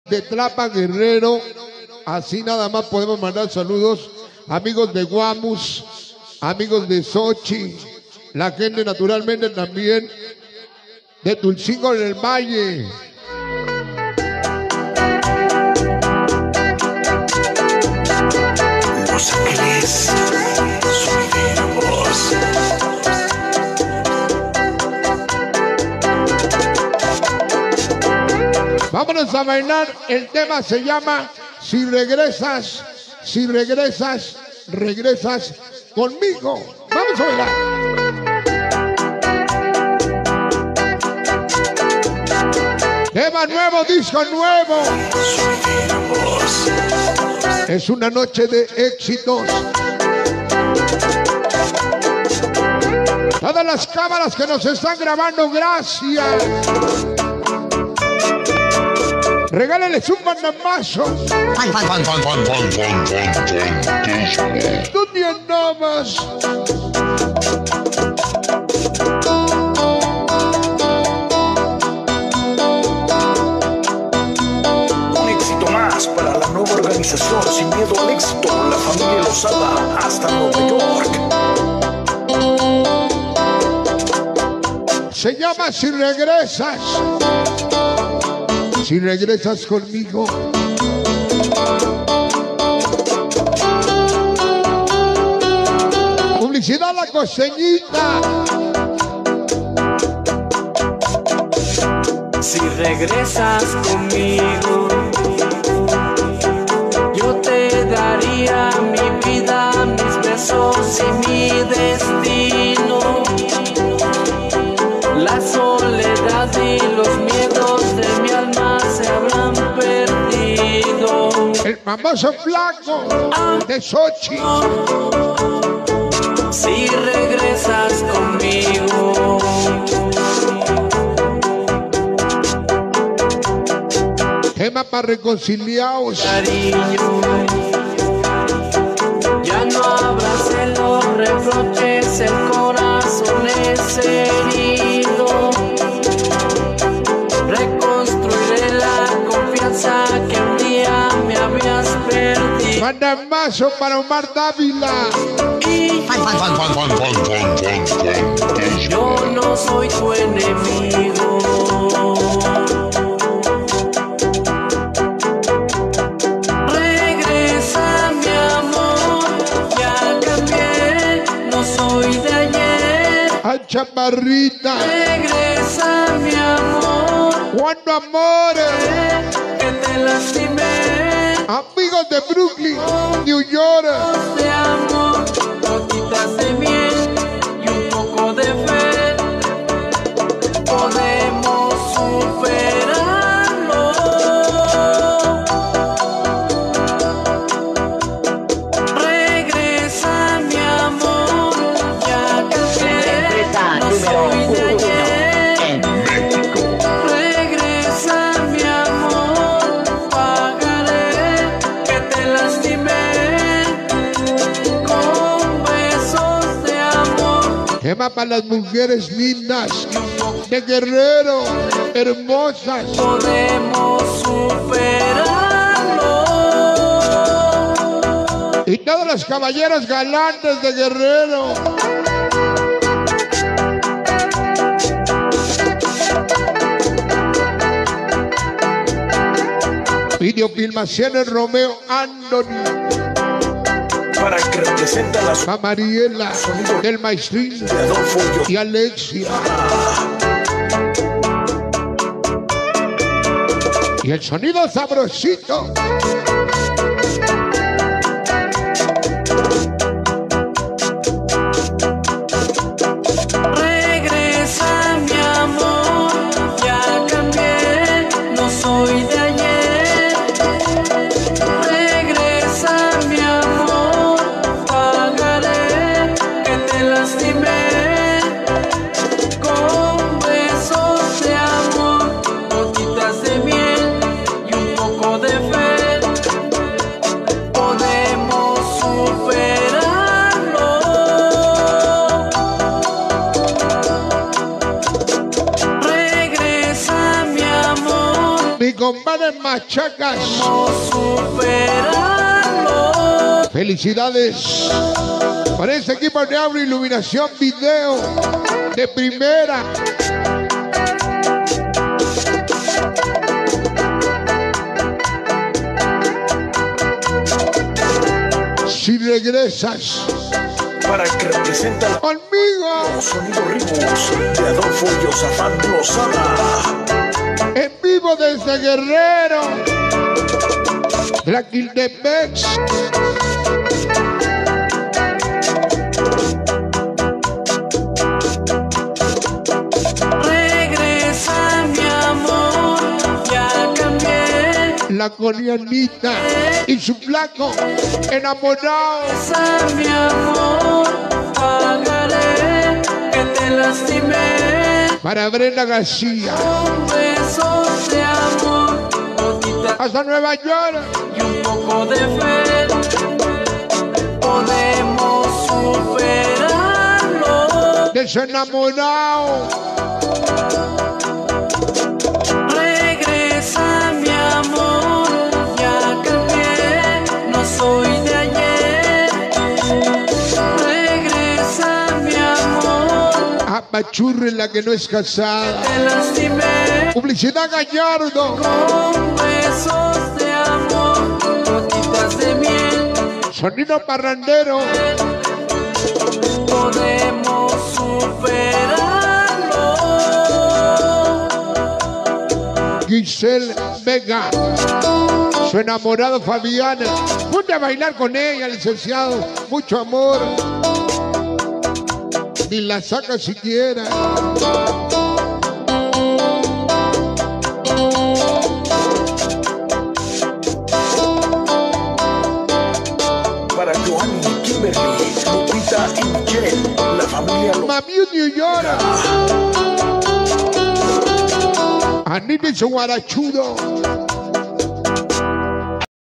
💜SONIDO FANTASMA🛑 LA BACHATA QUE SORPRENDIO A TODOS🔥CÉSAR JUÁREZ ASI TOCA // TLAQUILTEPEC GRO 2022
12 de octubre 2022